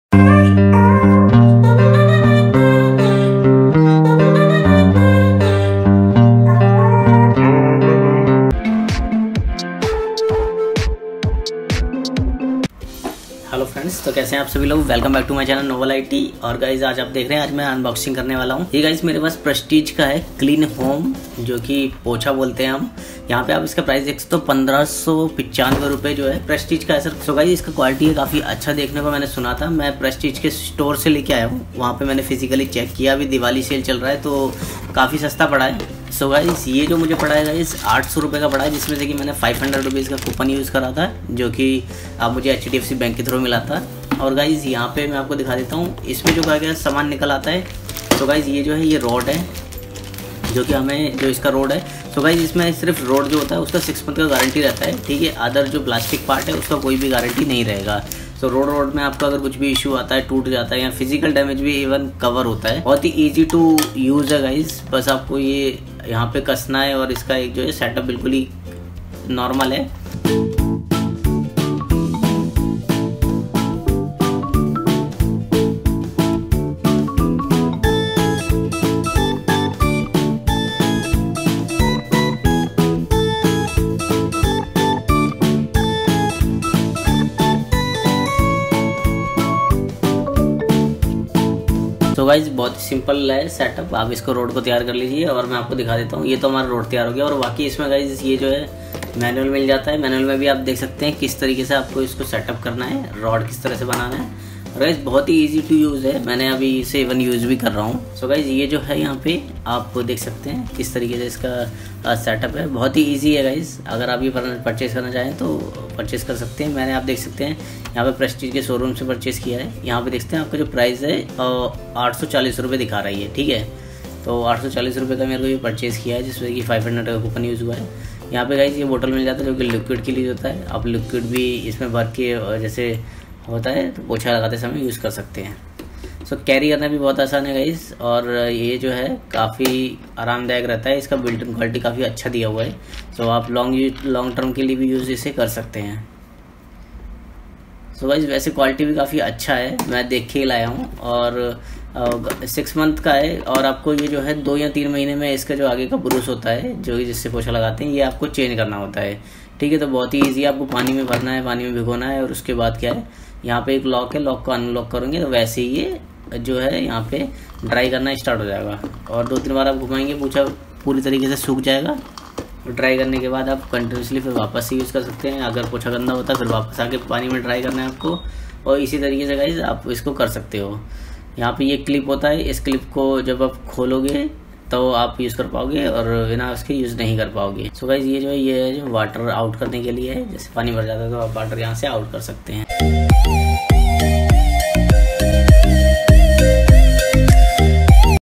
Unboxing of Prestige Clean Home Magic Spin Mop Deluxe PSB 800Rs ONLY .@Novel IT
Hi Friends,
In this video I am going to Unbox of Prestige Clean Home Magic Spin Mop Deluxe PSB 800Rs ONLY  .@Novel IT@Novel IT